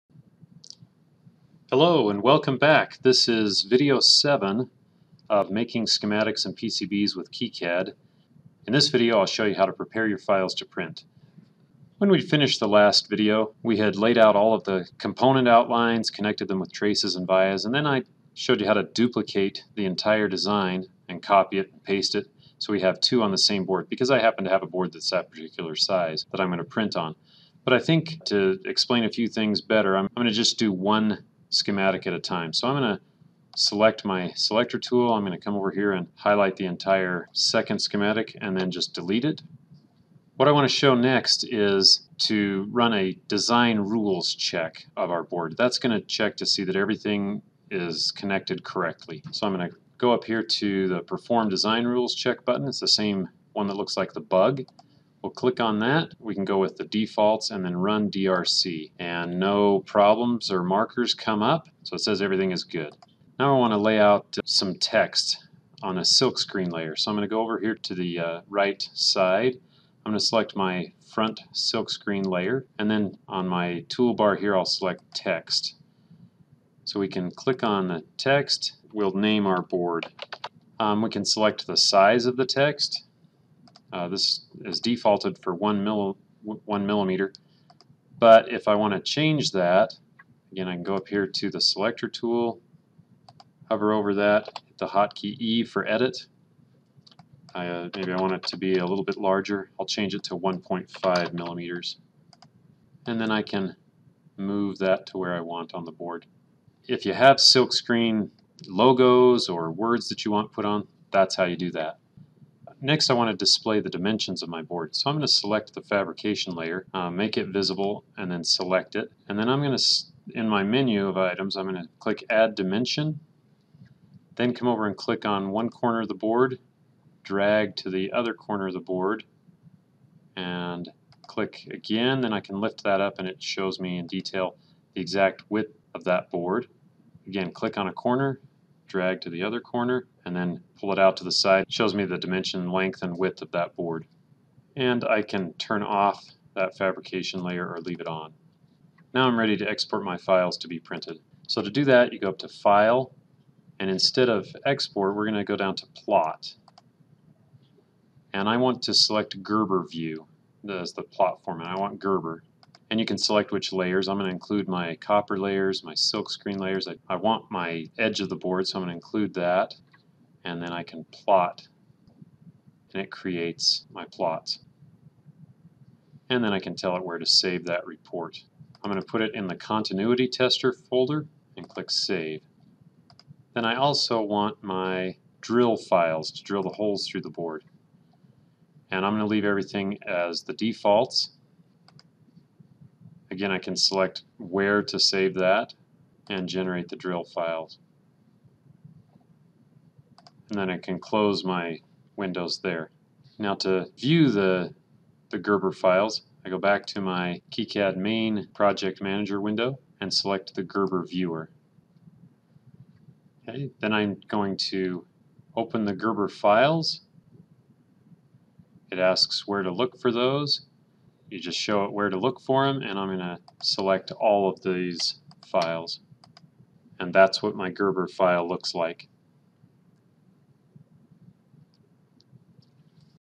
Making Schematics and PCBs with KiCad Part 7
teaches how to export gerber files from KiCad to print a PCB